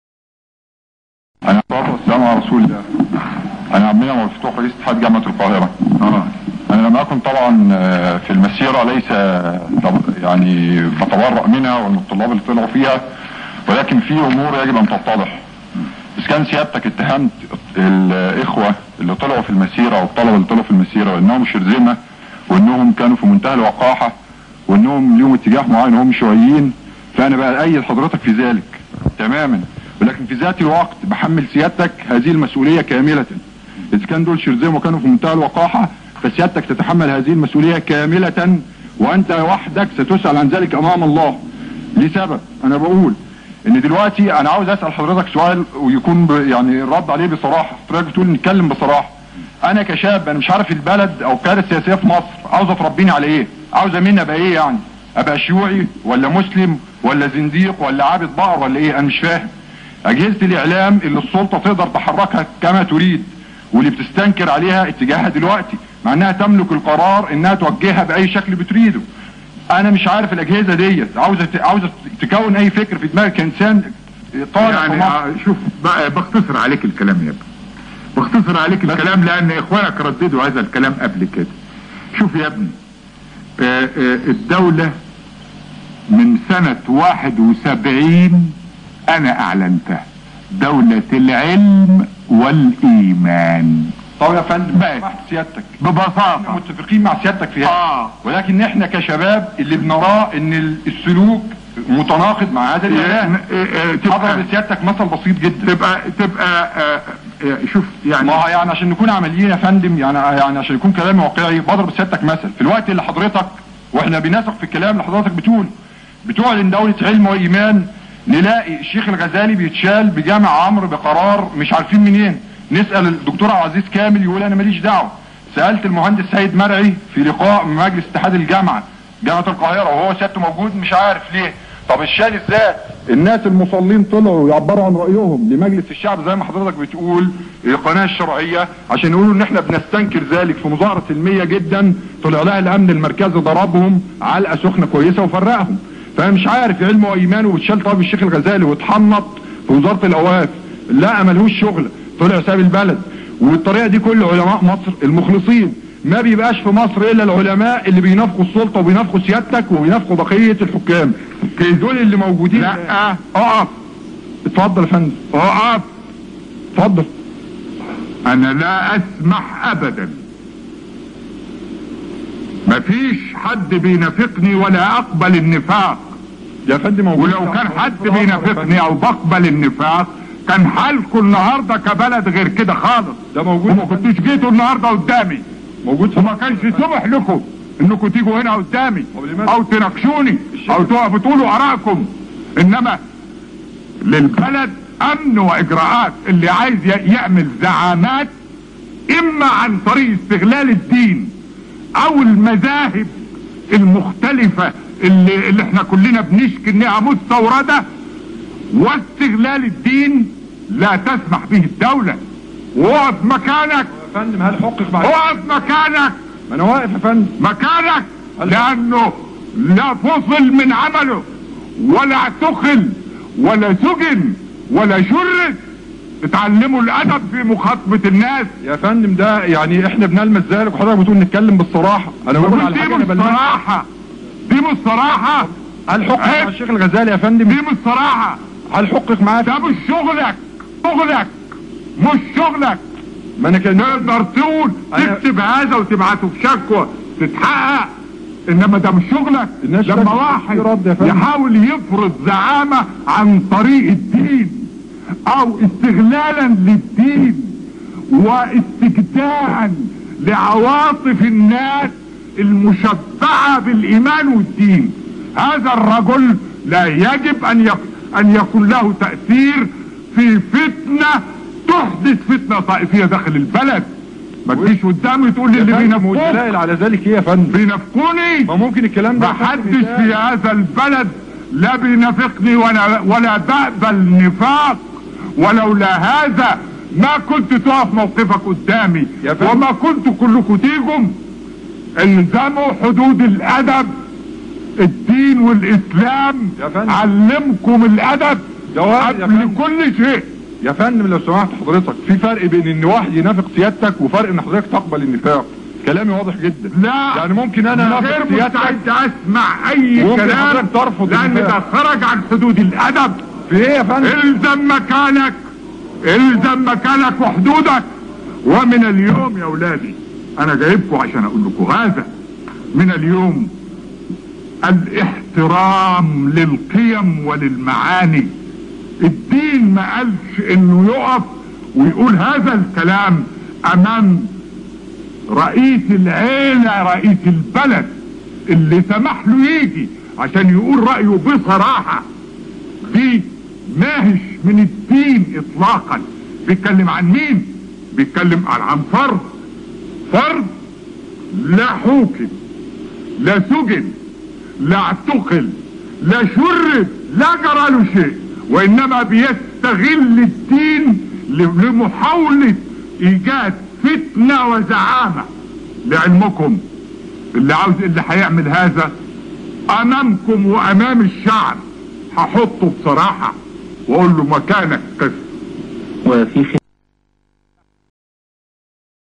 المواجهه التي هزت السادات
كلمة عبد المنعم ابو الفتوح للسادات ورد الرئيس السادات علية